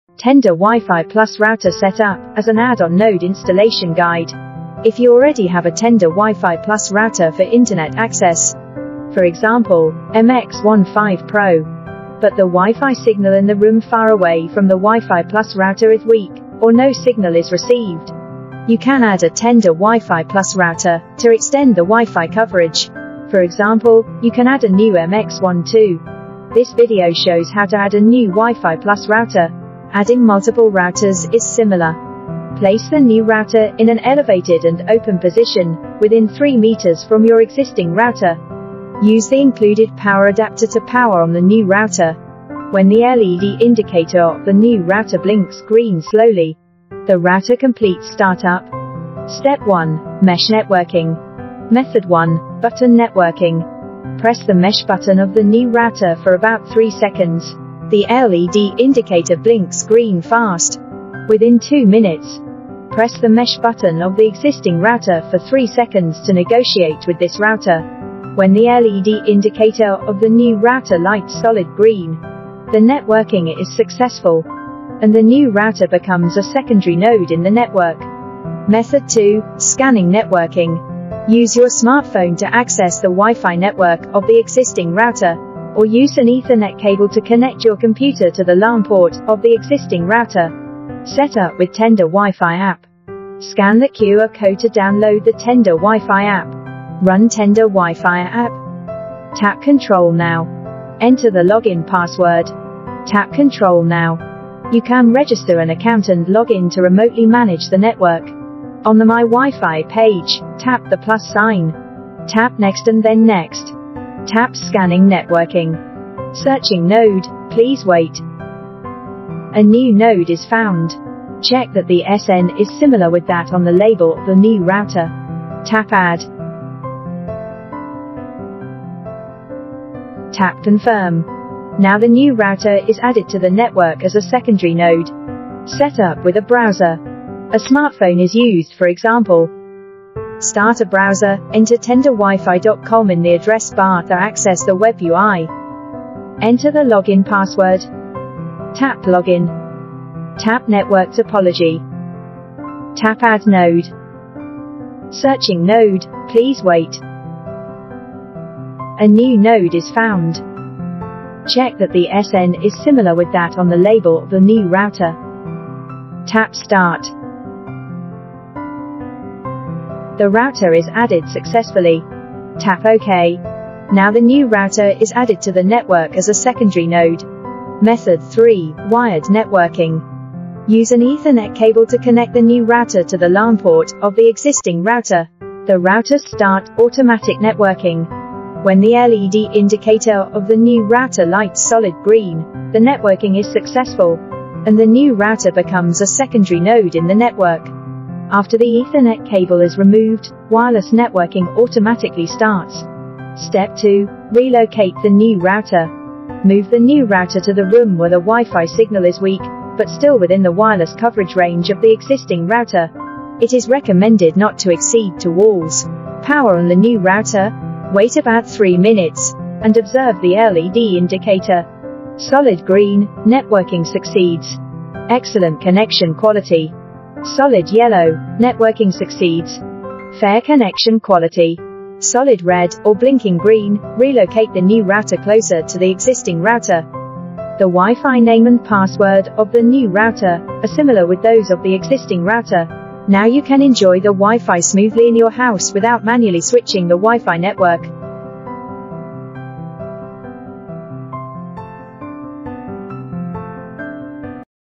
Tenda Mesh WiFi System Set Up Guide-How to add a new node for your wifi System
This video uses the products MX15 Pro and MX12 as examples to show you how to use different mesh routers for wireless networking mesh and wired networking mesh.
If you are interested in our Mesh WiFi System, you can search it on Amazon